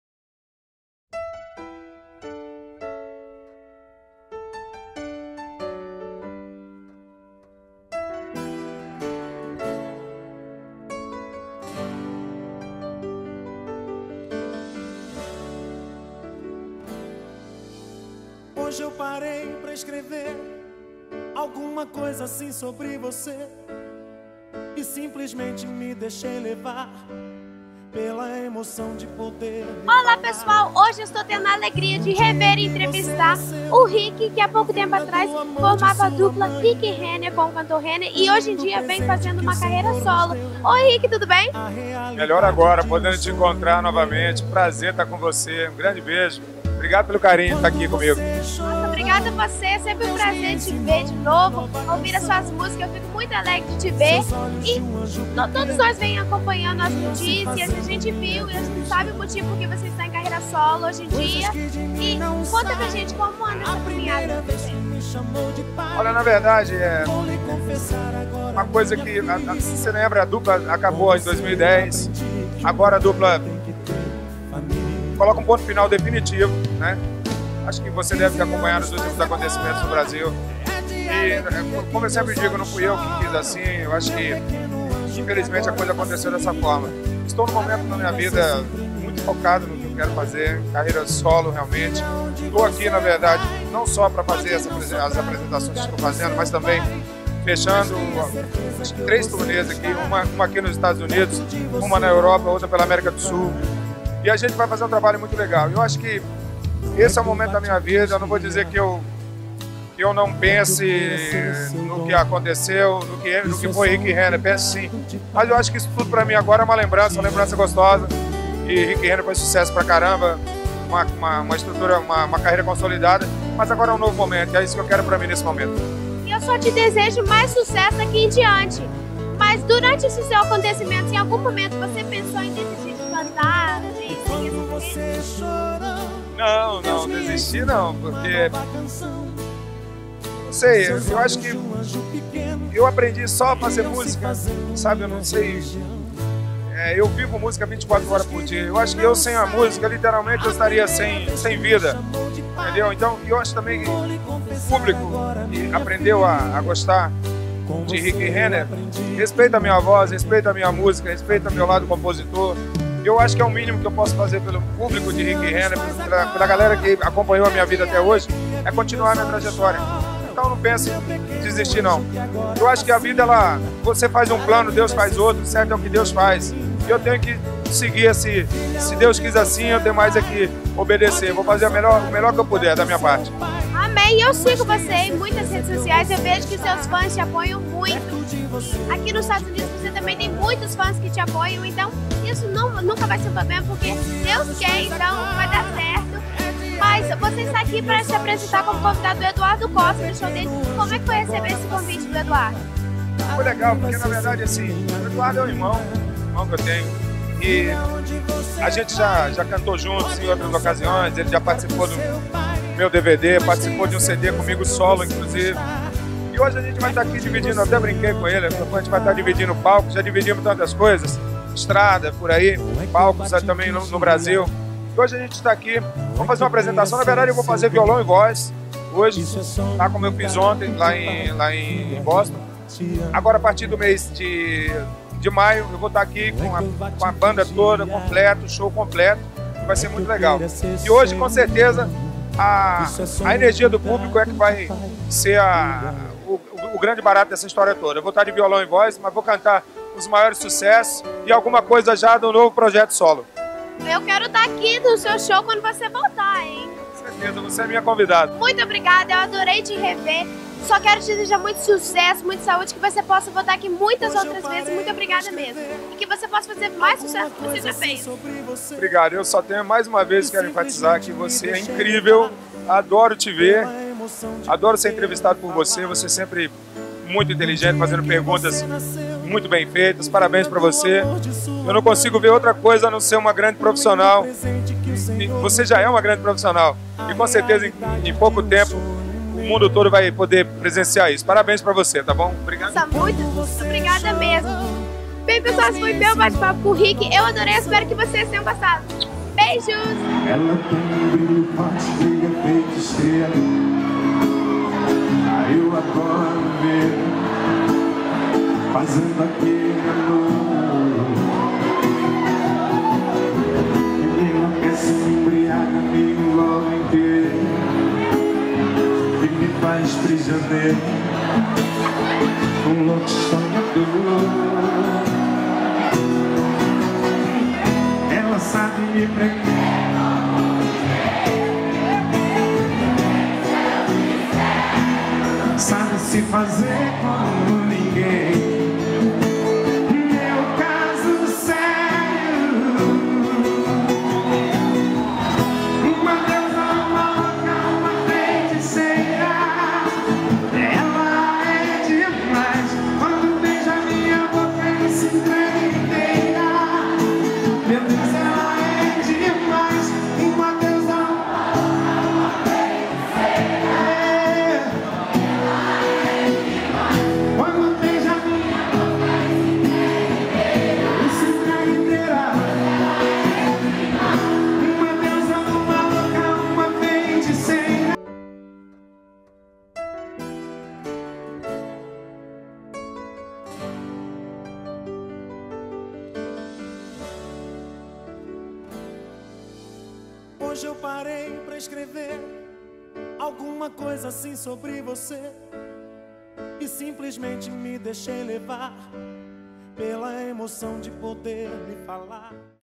VICKY SHOW ENTREVISTA O CANTOR RICKY SOLO
Depois de longos anos cantando junto com o Renner o cantor Rick inicia sua carreira solo e em convite do cantor Eduardo Costa ele esteve nos EUA para uma apresentação e Vicky esteve la conferindo tudo e batendo um papo com ele, confira.